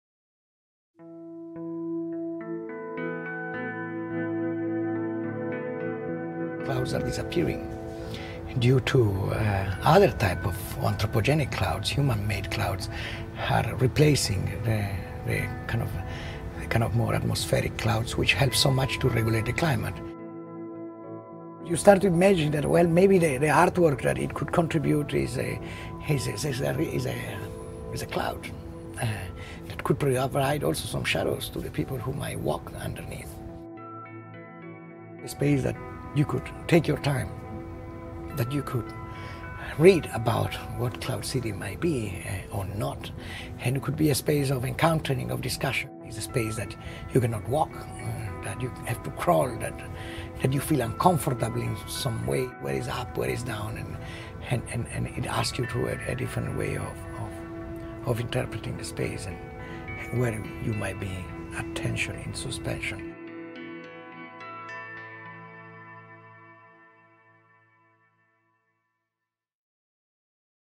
Interview Tomás Saraceno Cloud Cities Barcelona | Mirador torre Glòries
Discover what is behind Cloud Cities Barcelona at more than 130 meters high, in the dome of Torre Glòries. In his interview, Tomás Saraceno, its creator, explains what inspired him to create this work of art.

Descubre qué se esconde detrás de Cloud Cities Barcelona a más de 130 metros de altura, en la cúpula de la Torre Glòries. En su entrevista, Tomás Saraceno, su creador, explica qué lo inspiró a crear esta obra de arte.

Descobreix què s’amaga derrere de Cloud Cities Barcelona a més de 130 metres d’altura, a la cúpula de la Torre Glòries. A la seva entrevista, Tomás Saraceno, el seu creador, explica què el va inspirar a crear aquesta obra d’art.